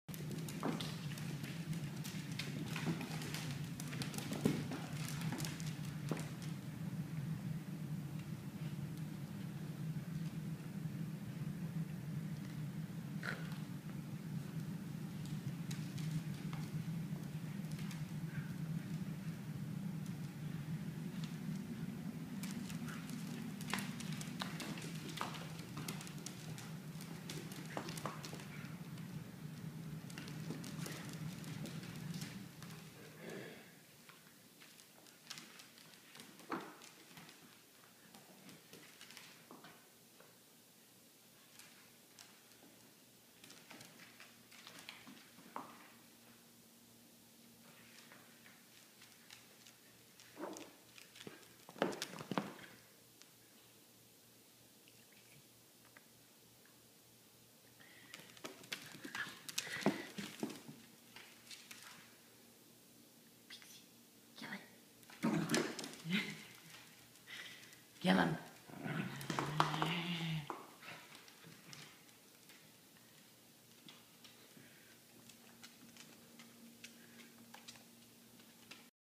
Corgi tug-o-war
Pixie and Poppet play tug of war